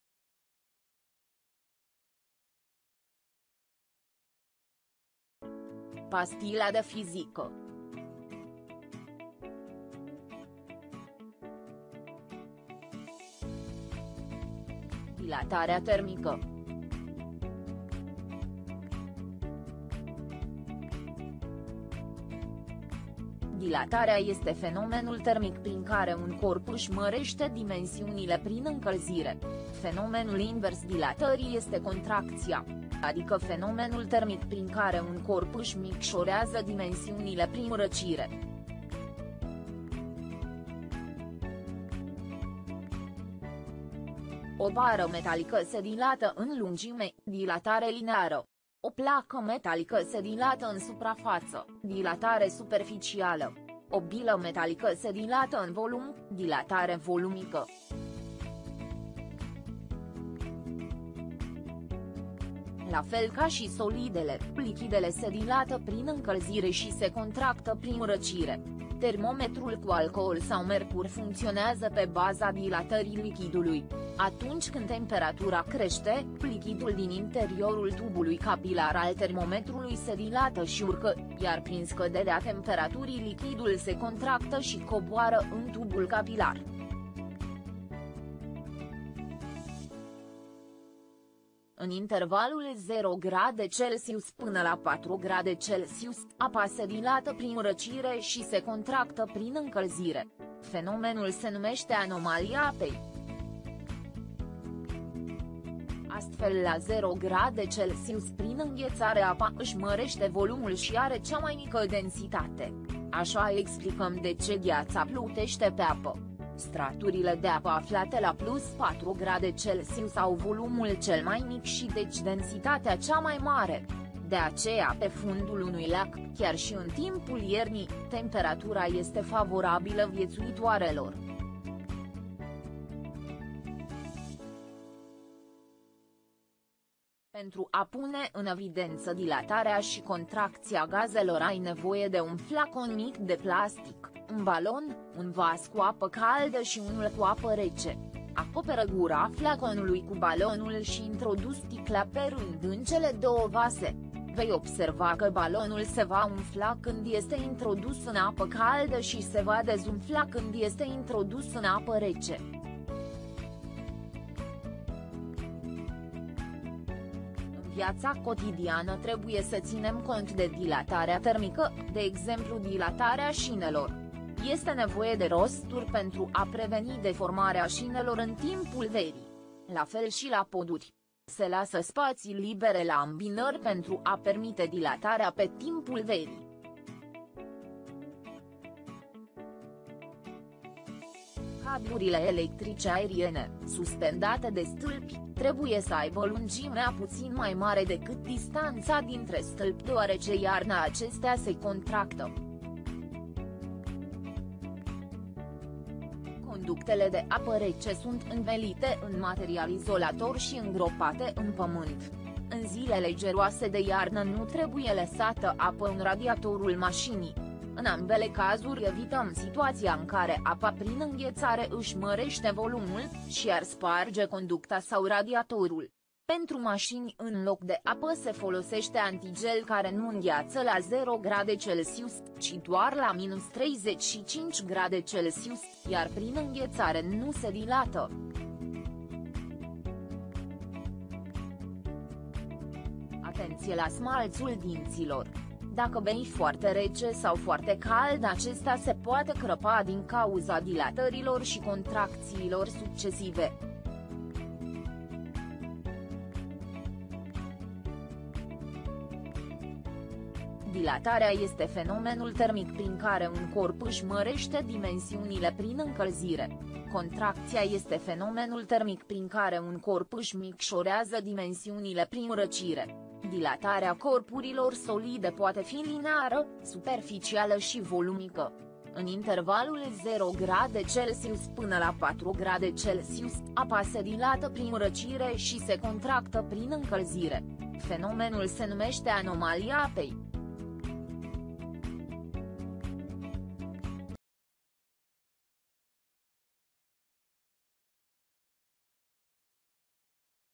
Dilatarea termică
Resursa educationala “Pastila de fizica” se doreste a fi un film scurt de 2-5 minute si care sintetizeaza elementele de baza necesare intelegerii notiunii abordate in video:
 definitii
 exemple
 formule
 imagini sugestive pentru intelegerea temei
 animatii sugestive pentru intelegerea temei
 aplicatie practica: realizarea unui experiment pentru punerea in evidenta a dilatarii si contractiei aerului
 infografic recapitulativ
Profesorul poate utiliza resursa ca element de fixare a cunostintelor in lectia de predare a notiunilor din capitolul de “Fenomene termice” sau intr-o lectie de recapitulare a capitolului.
Resursa a fost conceputa in asa fel incat orice elev sa o poata parcurge si singur acasa sau la scoala, dar aceasta poate fi parcursa si frontal la clasa sau in sistem online.
Profesorul poate oferi explicatii suplimentare elevilor care au nevoie de acestea sau pot permite altor elevi de a explica peer to peer colegilor informatiile, formandu-si astfel limbajul stiintific necesar.

Autor: Rodica Florina Gianina Moise